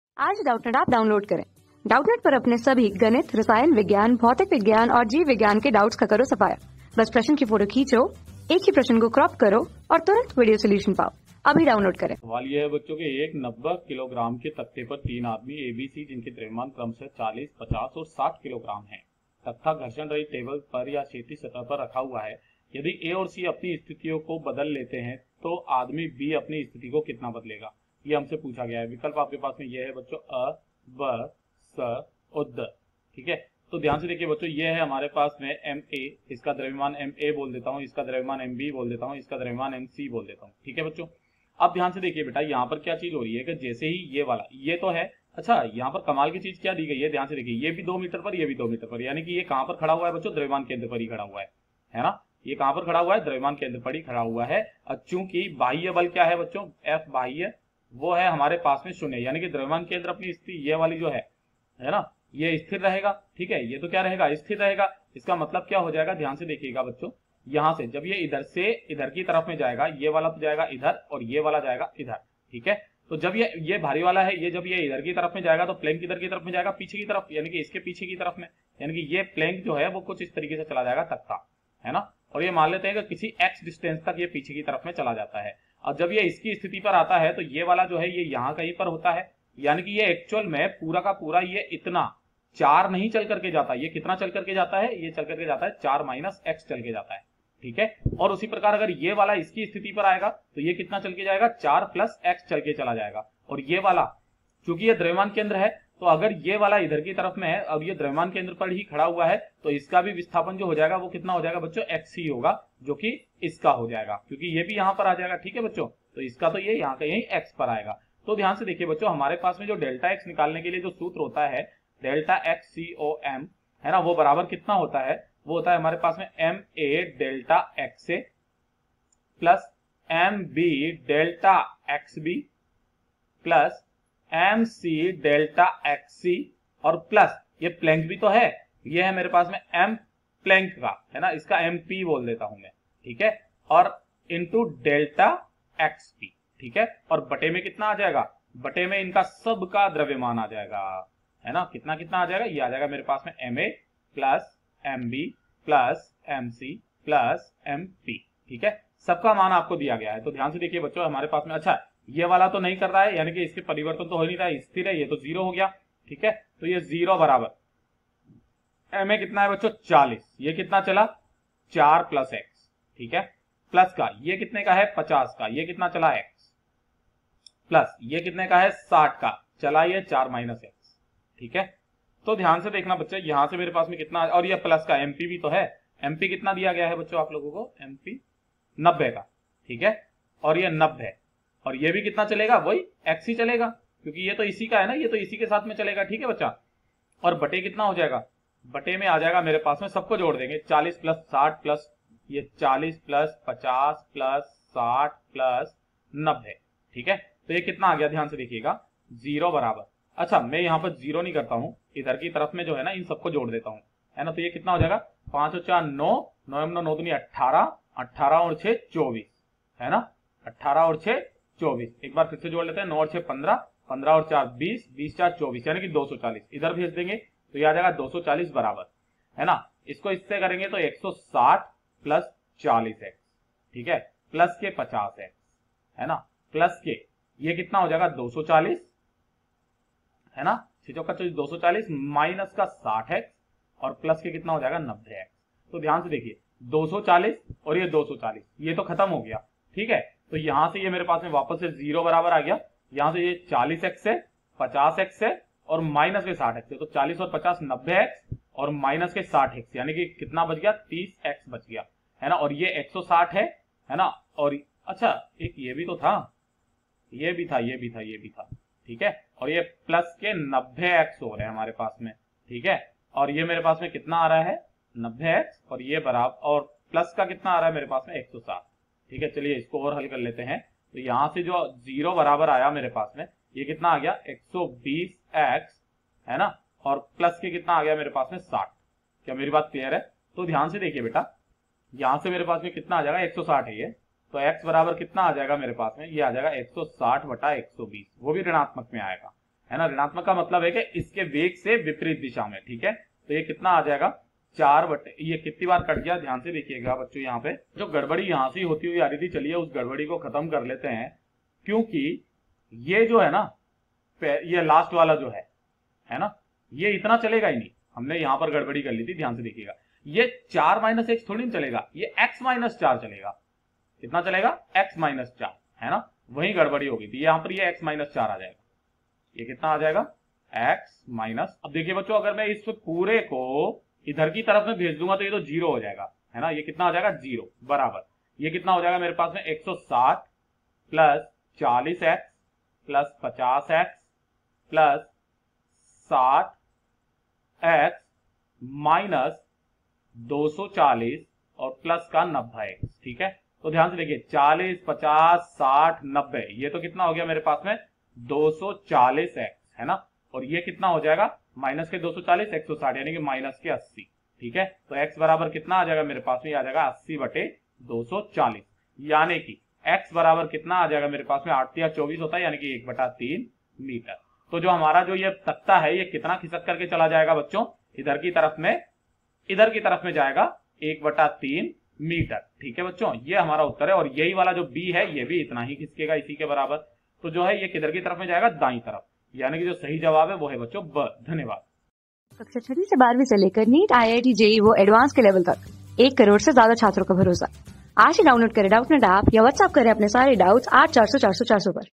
एक 90 किग्रा के तख्ते पर तीन आदमी A, B व C जिनके द्रव्यमान क्रमश: 40 किग्रा, 50 किग्रा तथा 60 क...
एक 90 किग्रा के तख्ते पर तीन आदमी A, B व C जिनके द्रव्यमान क्रमश: 40 किग्रा, 50 किग्रा तथा 60 किग्रा है, तख्ता घर्षण रहित क्षैतिज सतह पर रखा है। यदि A तथा C अपनी स्थितियों को बदल दे तो अदमी B अपनी स्थिति को कितना बदलेगा।
Class: 12
Subject: PHYSICS
Chapter: द्रव्यमान केन्द्र तथा संघट्ट
Board:IIT JEE

👉 Have Any Query? Ask Us.